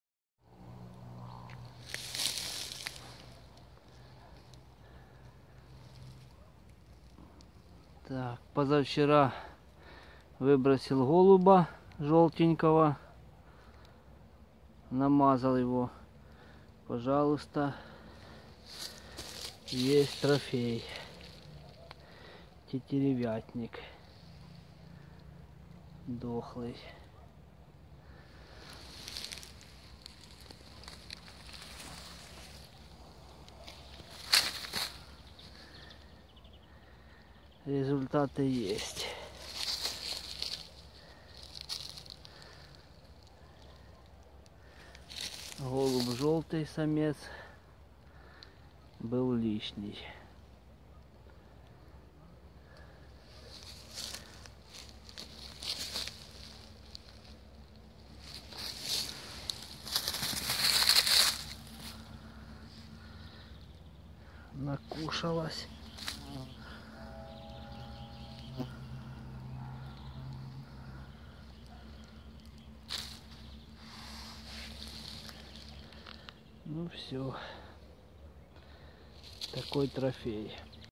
Ястреб и голубь.
Как получается у друзей ограничивать численость хищника.